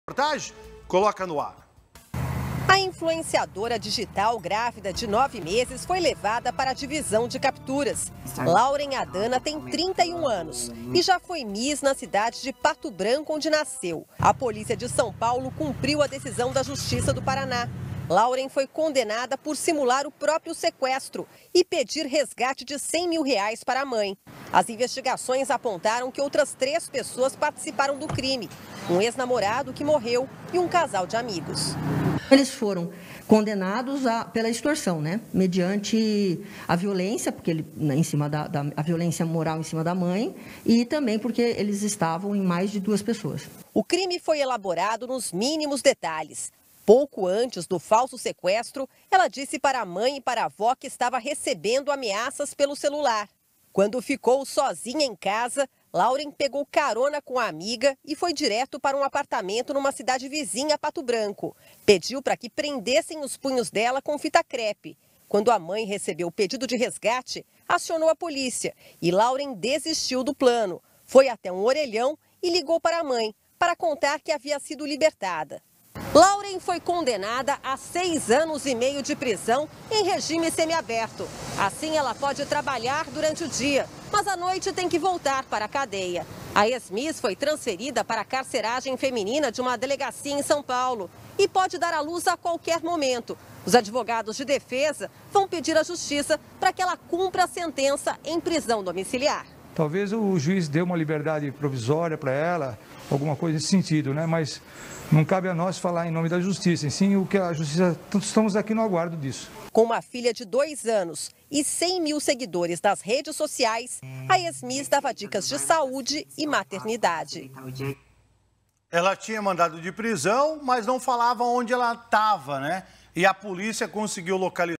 Influenciadora digital foi condenada por simular o próprio sequestro e extorquir a mãe
A moça foi condenada a 6 anos de prisão para cumprir a sentença em regime semi-aberto e pode dar a luz a qualquer momento.